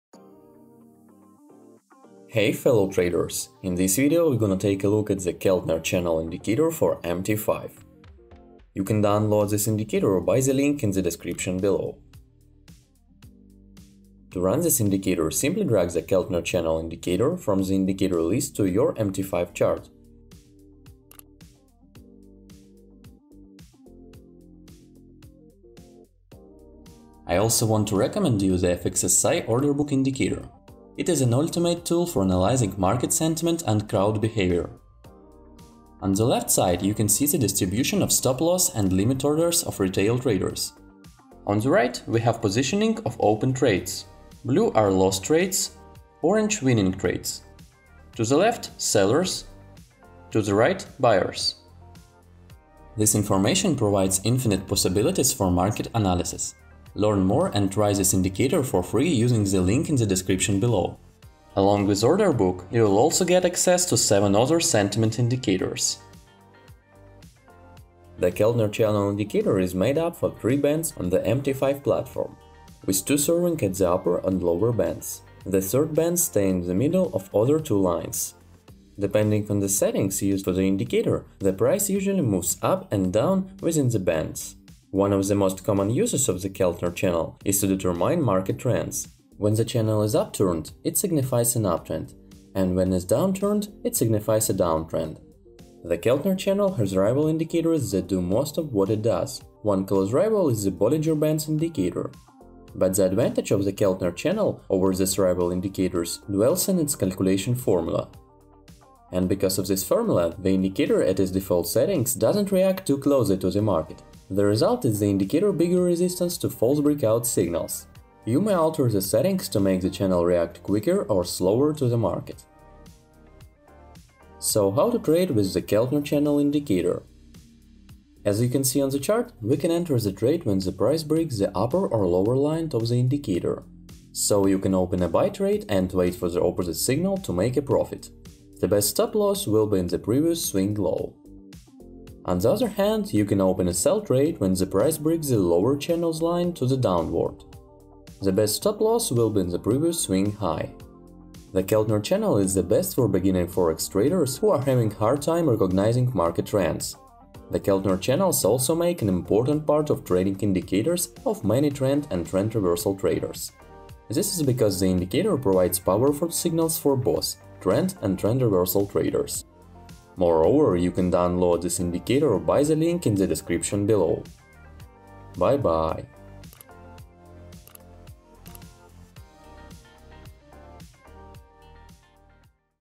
Keltner Channel Indicator for MT5 - FAST REVIEW
Keltner Channel Indicator for MT5 - Download FREE

The Keltner channel also makes up an important part of the trading indicators of many trend and trend reversal traders. This is because the indicator provides powerful signals for both trend and trend reversal traders.

Are you still trading without the Keltner Channel indicator? You are wasting your time.